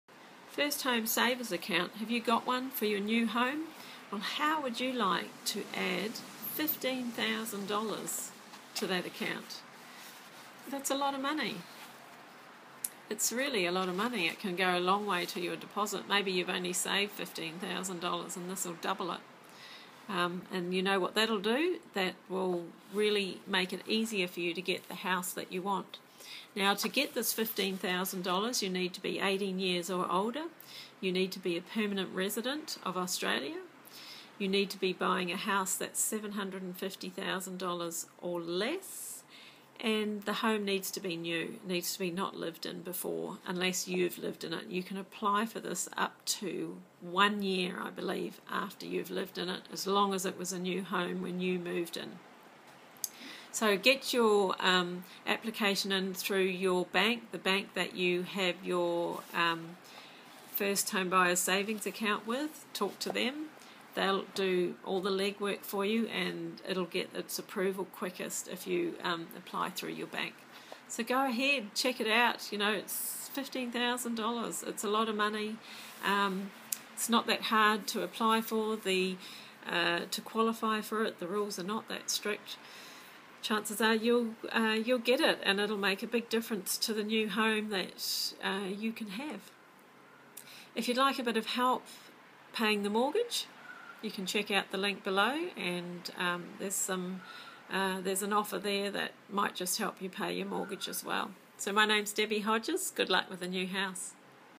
First Home Savers Account - Get Your Free $15,000 For Your New Home
First Home Savers Account - Get Your Free $15,000 For Your New Home and get into your new home sooner.
If you didn't already know that you may qualify to receive $15,000 to add to your first home savers account then you do now - you are welcome!
This is a great boost to getting into your first home so make sure you get it.
Let your mortgage lender organise it for you, it's quicker and easier this way.
It you are
-18 years or older
- an Australian permanent resident
- Buying a NEW home
- New home value must be $750.000 or less
- and have never owned a home in Australia before
then you should qualify.

Hope this is some help to you and it gets you into your new home sooner.
If you would like to make extra money or even replace your current income, then follow the link above to find out how.